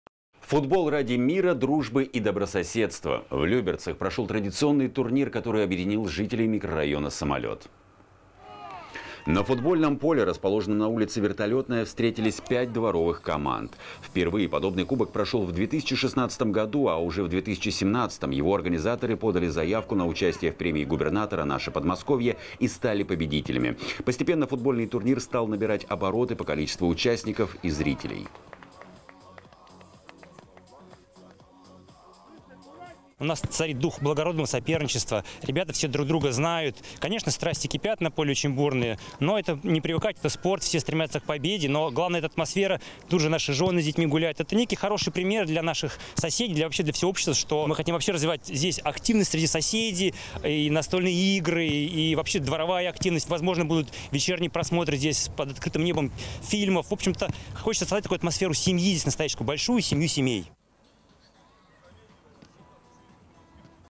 Играй в футбол ради мира
Футбол ради мира, дружбы и добрососедства. В Люберцах прошел традиционный турнир, который объединил жителей микрорайона «Самолет».

На футбольном поле, расположенном на улице Вертолетная, встретились 5 дворовых команд. Впервые подобный кубок прошел в 2016 году, а уже в 2017-м его организаторы подали заявку на участие в премии губернатора «Наше Подмосковье» и стали победителями. Постепенно футбольный турнир стал набирать обороты по количеству участников и зрителей.

Дмитрий Самко, председатель региональной общественной организации «Федерация за всеобщий мир»:
«У нас тут царит дух благородного соперничества, ребята все друг друга знают, конечно, страсти кипят на поле очень бурные, но это спорт, все стремятся к победе, но главное атмосфера, тут наши жены с детьми гуляют. Это некий хороший пример для наших соседей, для всего общества. Мы хотим развивать здесь активность среди соседей, настольные игры, дворовая активность, возможно будут вечерние просмотры под открытым небом фильмов. Хочется создать такую атмосферу семьи настоящую, большую семью семей».